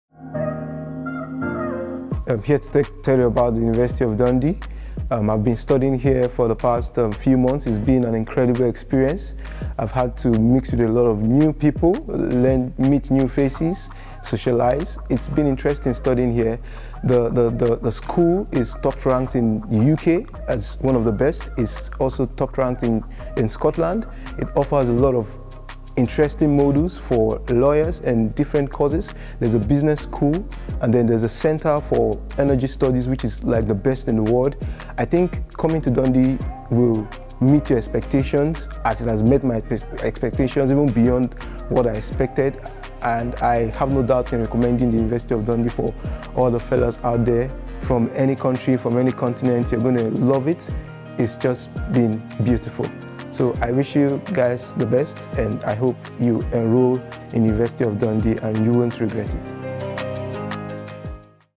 University of Dundee | Law | Welcome to the University of Dundee
David Okorogba, an LLM Law, Banking and Finance student welcomes you to the University of Dundee.